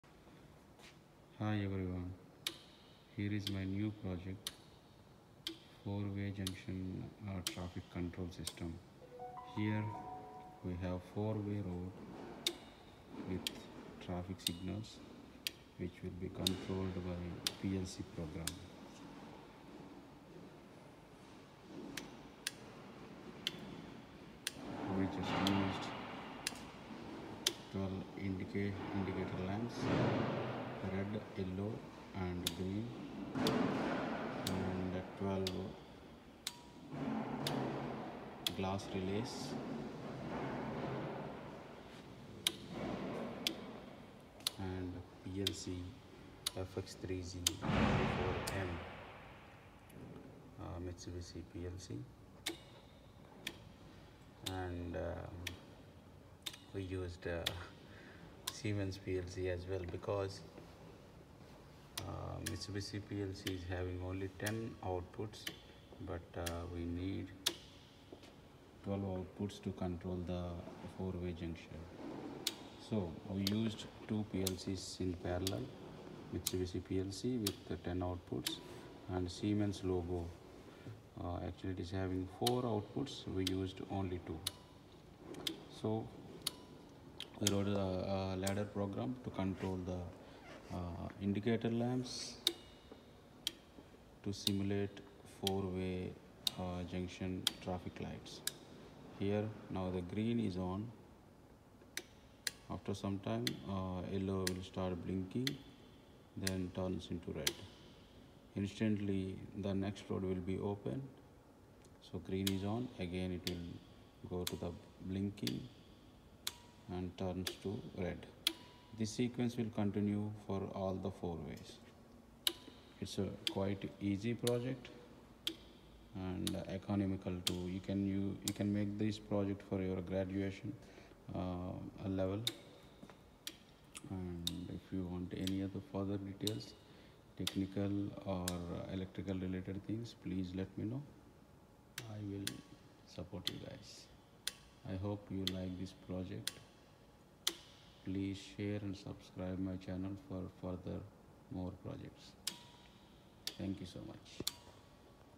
PLC based project: 4 way Junction traffic control system with PLC's
It is a project idea for engineering graduates. PLC programming is is used to control the traffic lights through glass relays. ladder program consists of timers , counters and internal memory coils and so on.

control wiring and power wiring is also required to produce this kind of projects. In further it can be improved to smart traffic system by using smart sensors